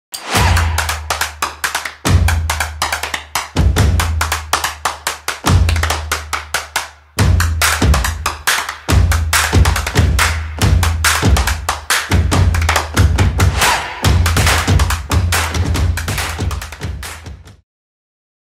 The New Normal Teaser I DTPNETID
Selamat berakhir pekan teman DTP!
Penting bagi kita untuk mengetahui cara beradaptasi yang baik dan benar pada masa transisi new normal saat ini.
Simak terus di akun kami untuk mengetahui lebih lanjut.